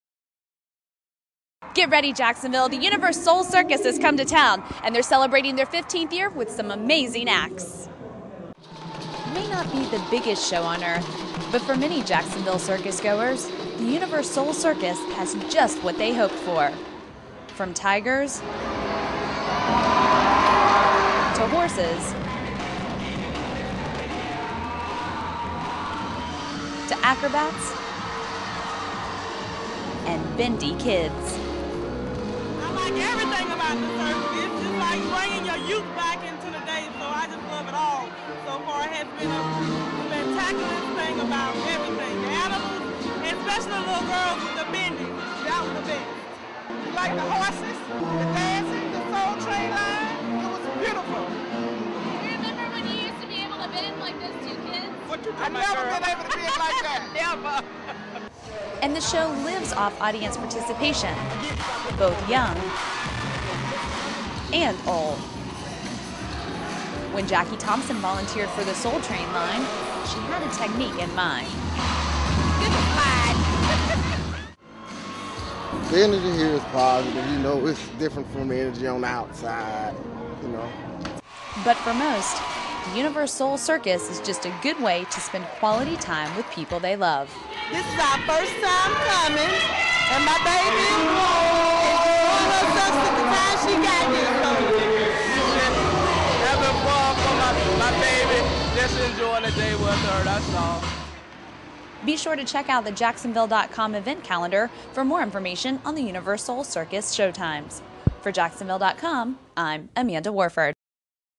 UniverSoul Circus
The UniverSoul Circus has come to Jacksonville and is celebrating its 15th year with some amazing acts.